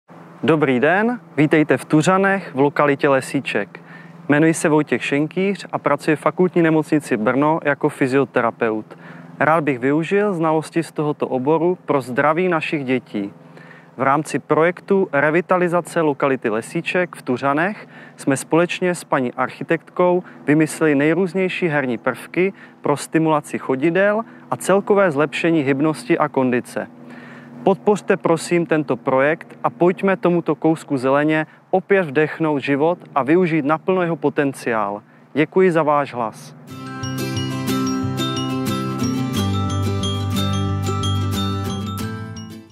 Revitalizace lokality Lesíček
Jeden z finálových projektů Dáme na vás. Líbí se vám nápad pana Vojtěcha Šenkýře pro lepší Brno? Tak pro něj hlasujte do 29. listopadu 2019 na Vítězné nápady zrealizujeme.